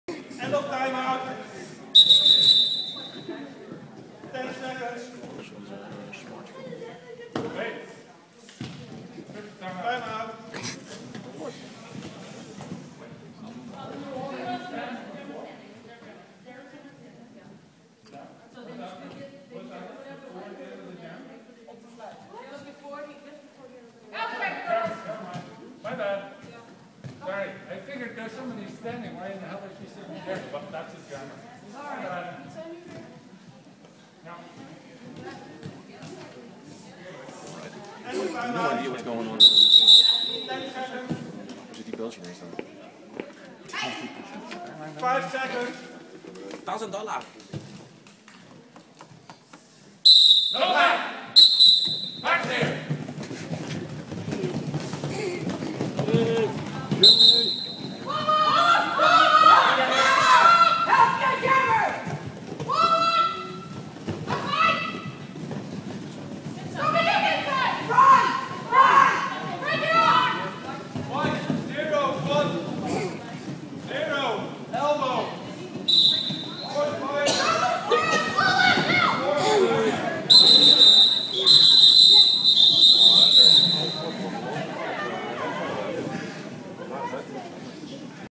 Weird Jam (Is it Jelly?)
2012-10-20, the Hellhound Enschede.
Eastside Rock'n'Rollers versus Death Row Honeys